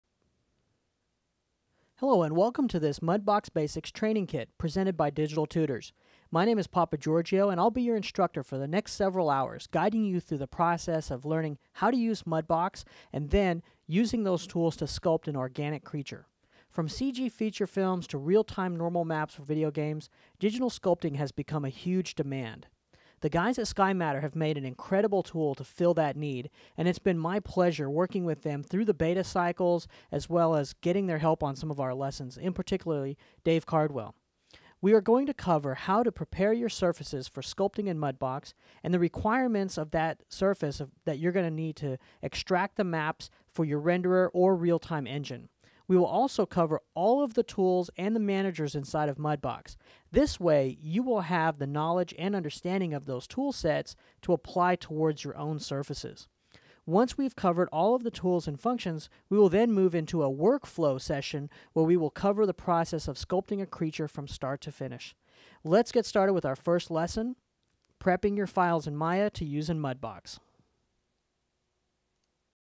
Project Overview of Introduction to Mudbox Training
, your home for video-based training. We have over 600 free video tutorials and add new ones every week!

Easily learn Mudbox and an array of time-saving modeling and sculpting techniques that are being used for Film and Games. Contains over 3.5 hours of project-based training. Great for new and beginning artists.